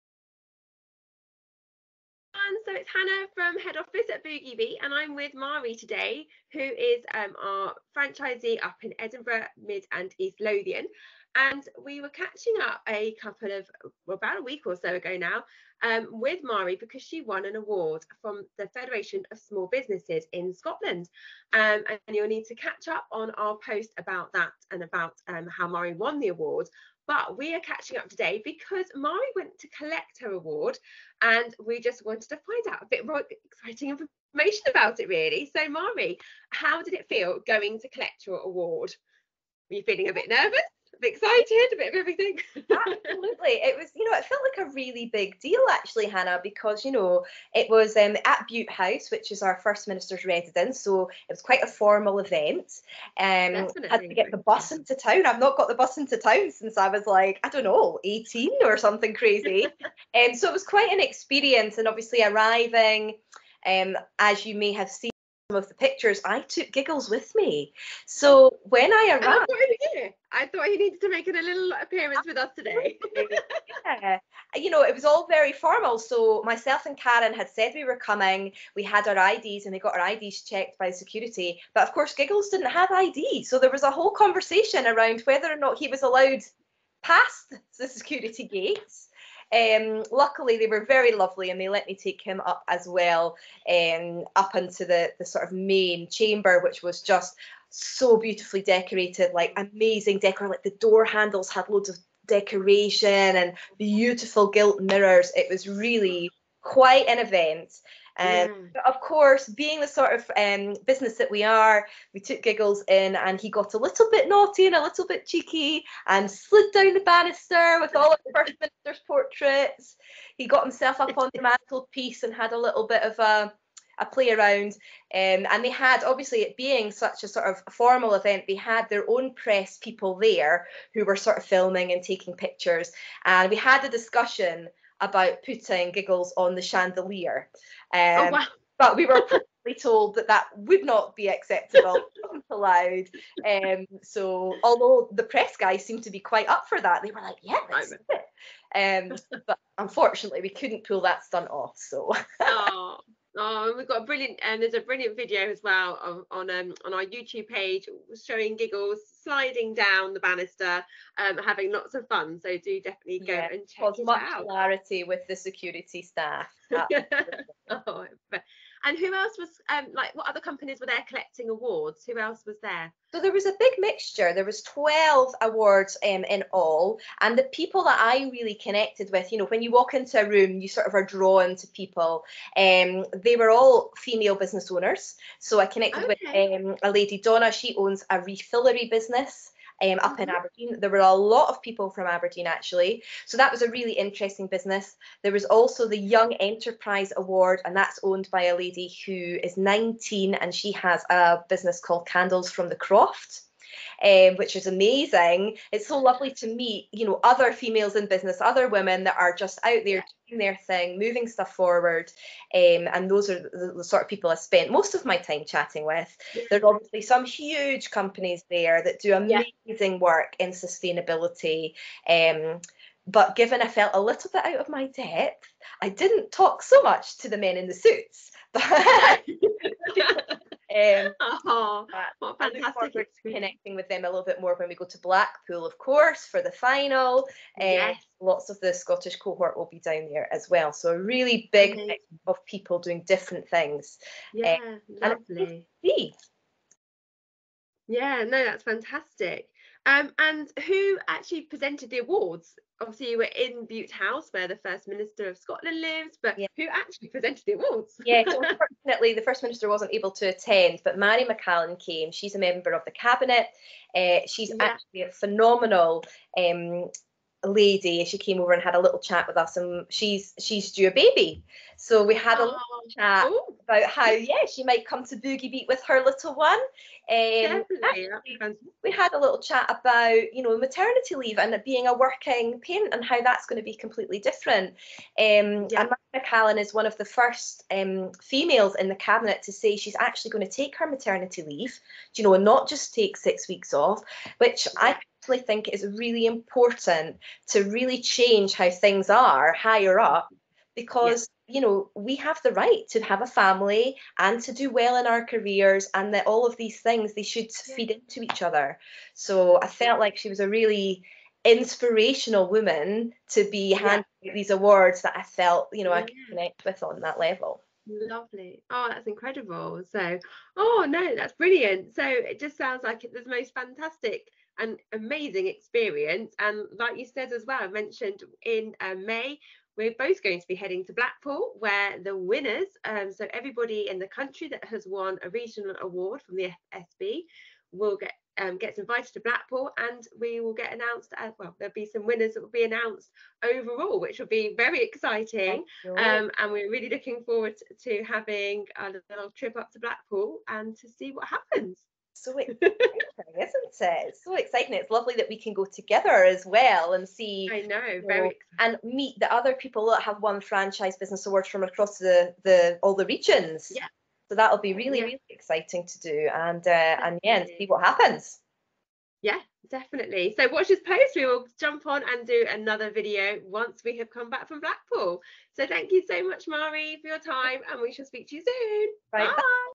Mairi collecting her FSB Award
Listen to what happened when Mairi from our Boogie Beat Music and Movement franchise in Edinburgh, Mid and East Lothian collected her FSB (Federation of Small Business) Franchise award.